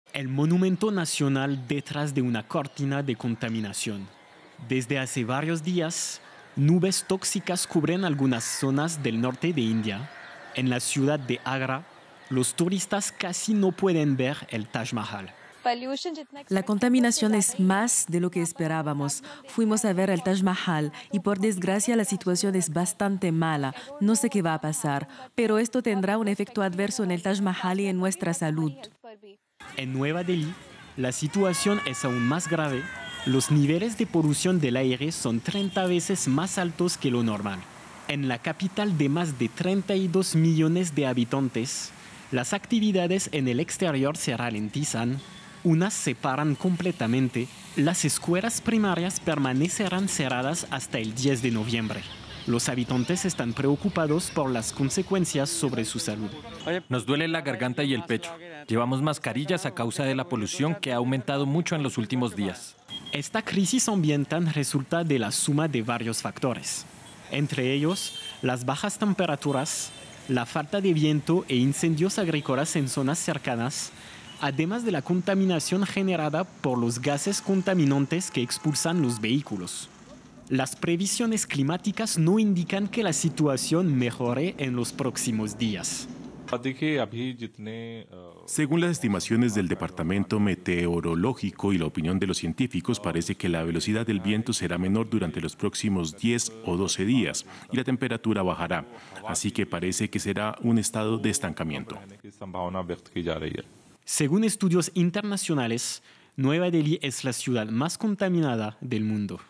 India: polución en Nueva Delhi es 30 veces mayor de lo recomendable por la OMS
La polución en Nueva Delhi, ciudad india con unos 32 millones de habitantes, ha alcanzado niveles alarmantes, superando más de 30 veces lo recomendable según la Organización Mundial de la Salud. Una situación que ha llevado a las autoridades a extender el cierre de escuelas primarias hasta el próximo 10 de noviembre, con la opción de realizar clases en línea para los cursos de secundaria.